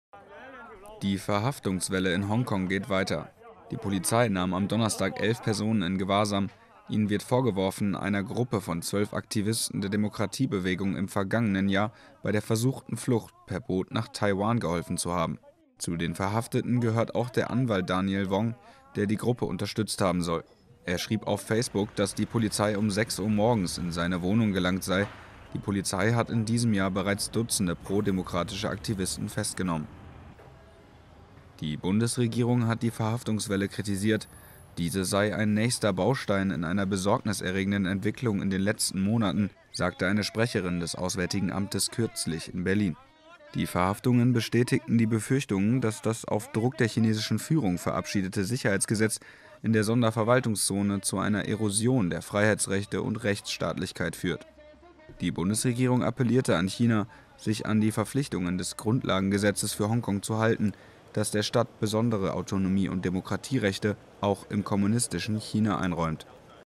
Erneut Verhaftungen von pro-demokratischen Aktivisten in Hongkong
Die Hongkonger Polizei hat am Donnerstag elf Personen verhaftet. Sie wirft den Beschuldigten vor, einer Gruppe von Aktivisten der Demokratiebewegung bei der versuchten Flucht per Boot nach Taiwan geholfen zu haben.